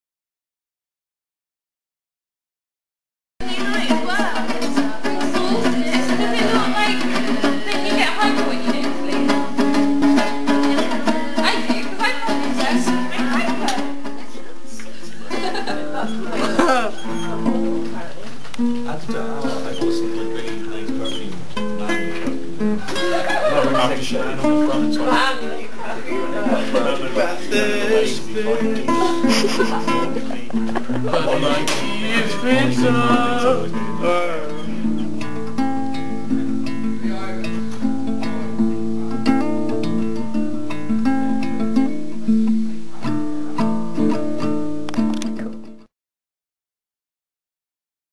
Last Days of my Spanish Guitar
Various people at college strumming along :)